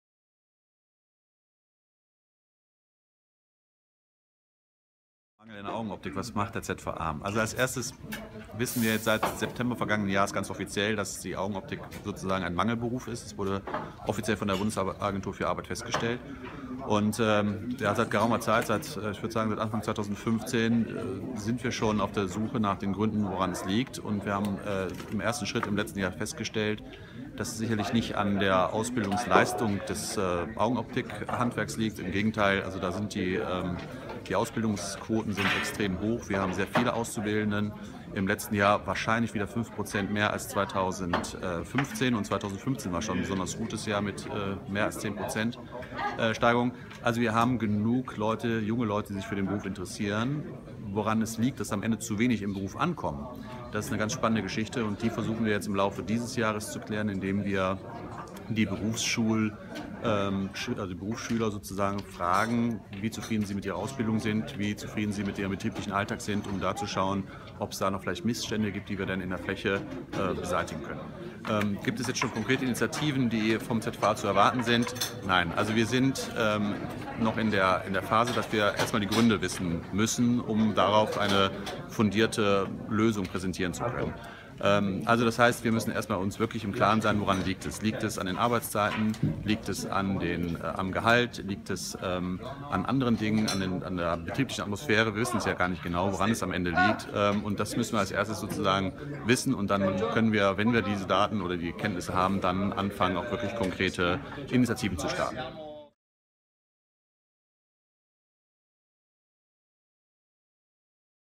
eyebizz Fachkraeftemangel Augenoptik
Fachkräftemangel: Brennendes Thema in der Augenoptik ein Interview mit Dr. Jan Wetzel, ZVA Geschäftsführer von unserer Chefredakteurin Christine Höckmann